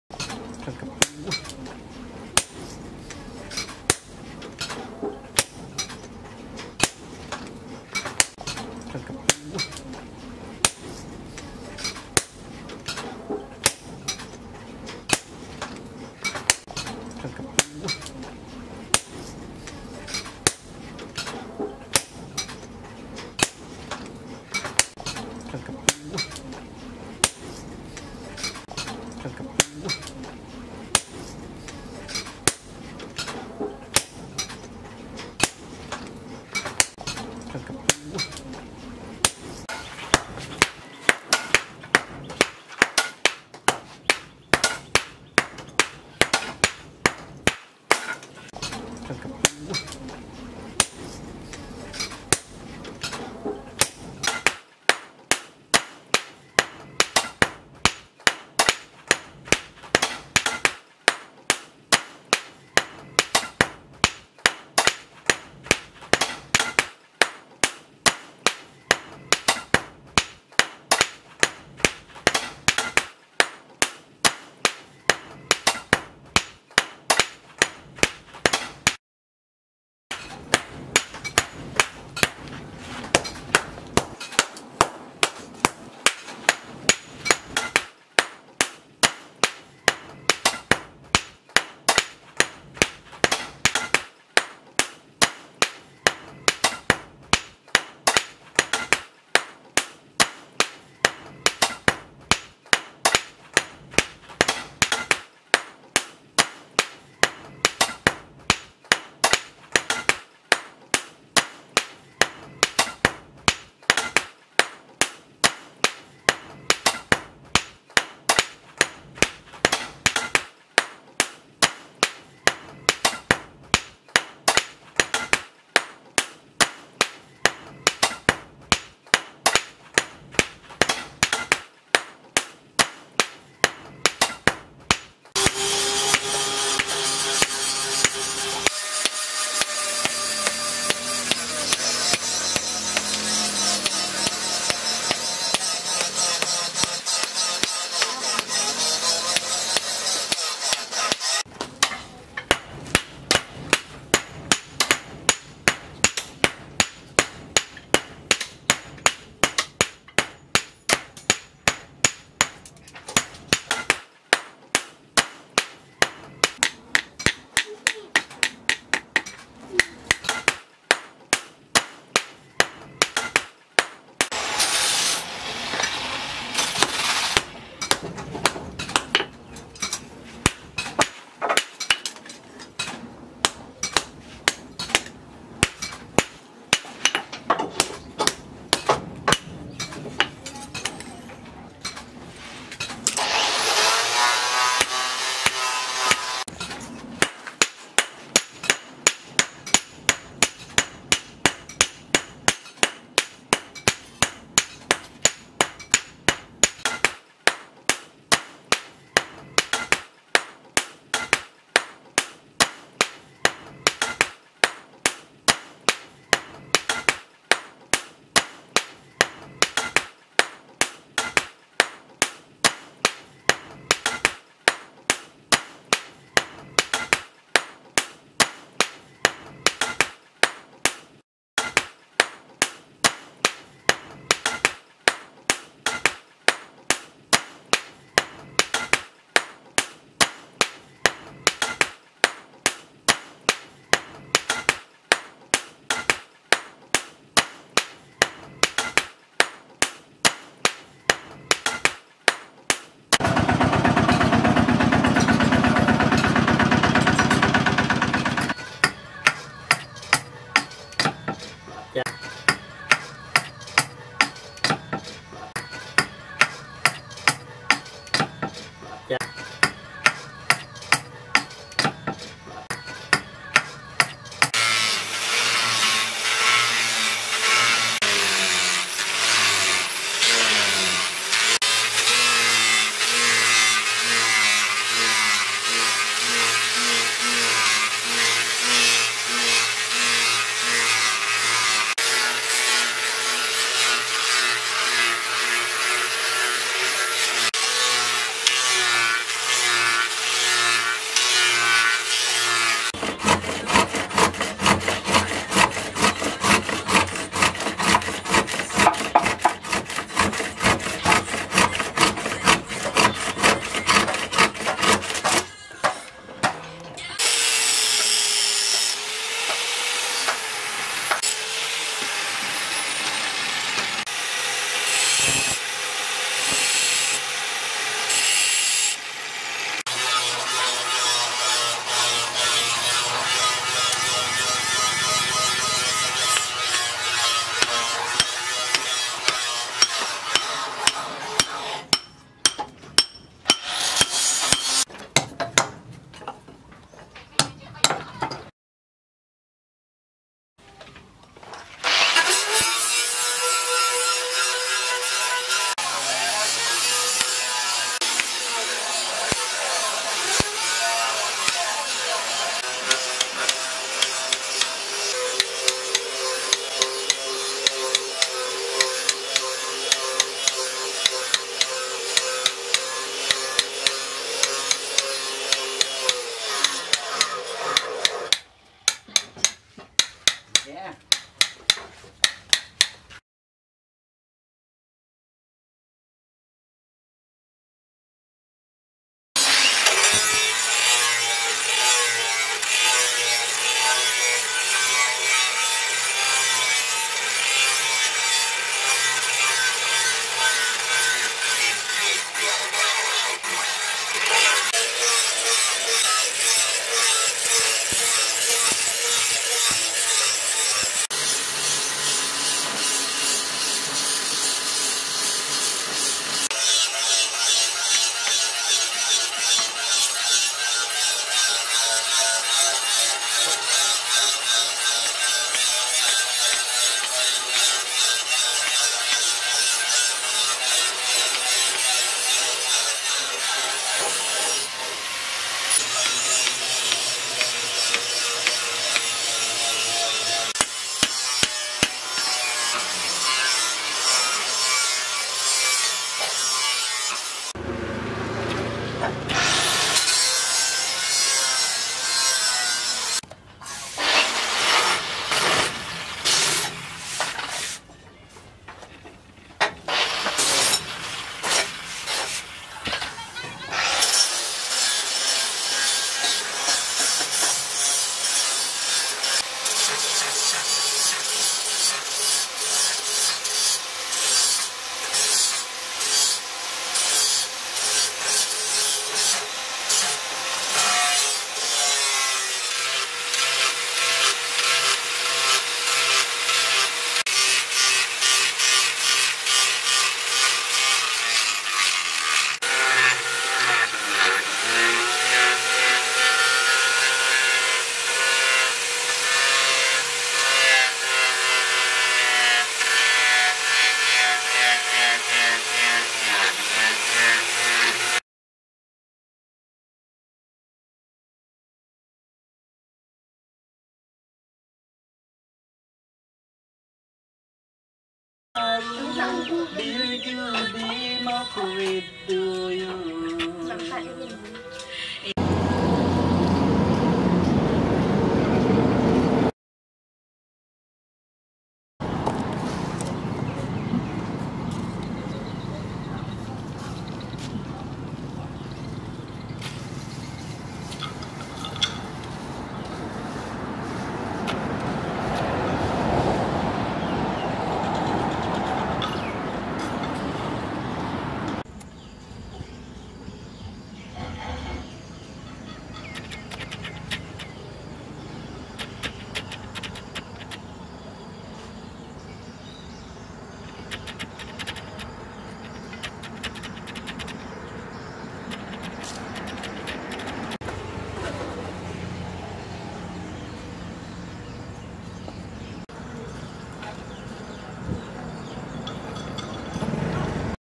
Making a beautiful knife called (Kambit Bontoh)
Published 26,07,2020
Hello everybody! welcome to my original videos channel. In this video you will see how blacksmith turns this kind of steel into a new beautiful useful thing in our daily lives. Thanks so much for your support and watching this videos.